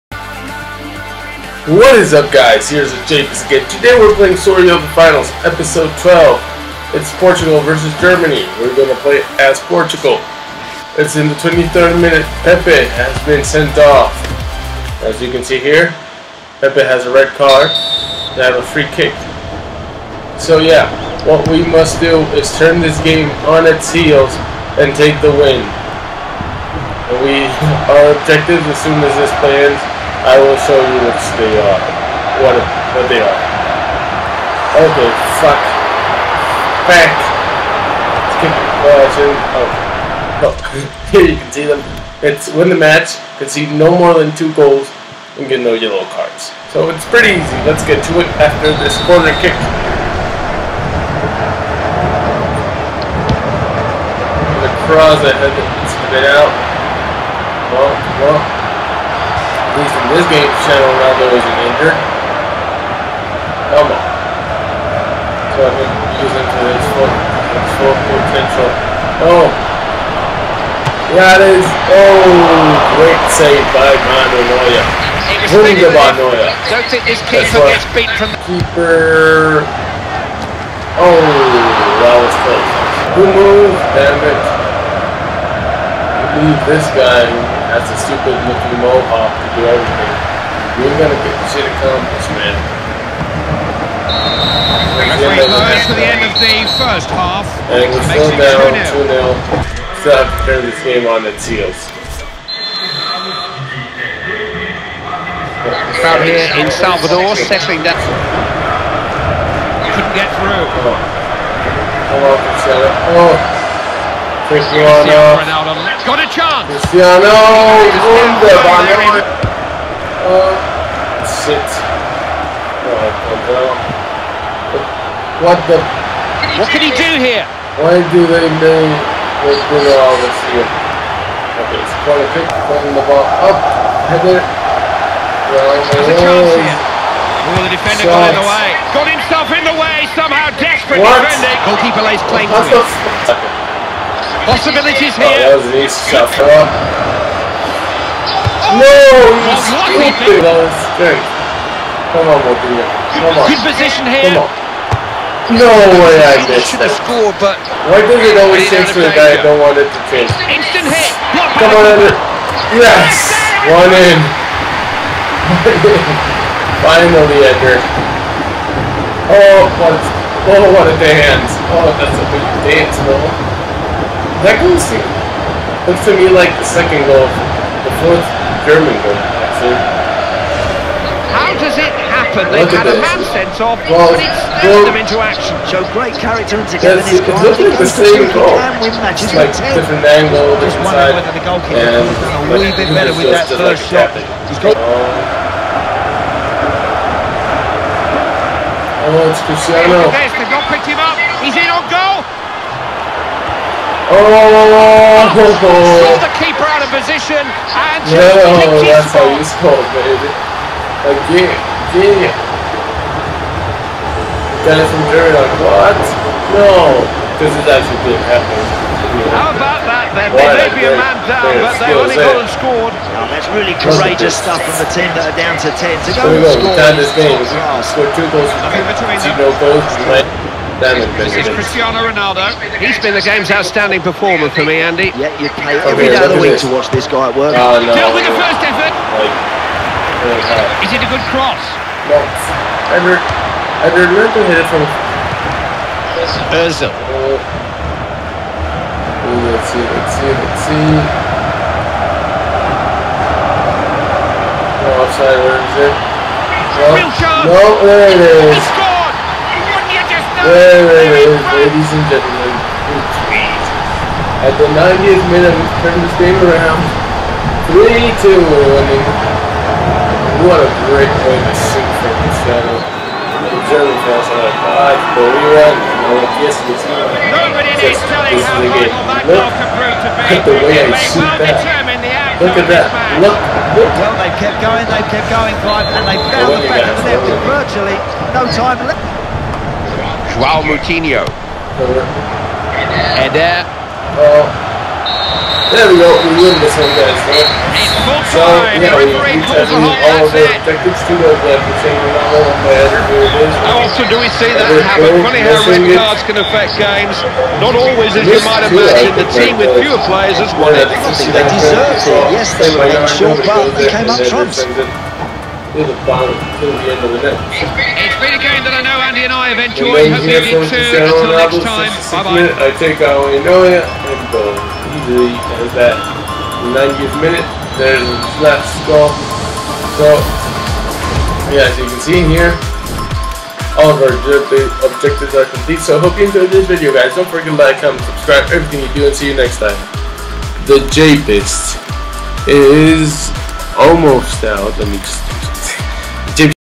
STORY OF THE FINALS EP.12 GERMANY VS PORTUGAL
Germany are 2-0 up near the second half. With one man down can CR7 and Portugal make a heroic comeback?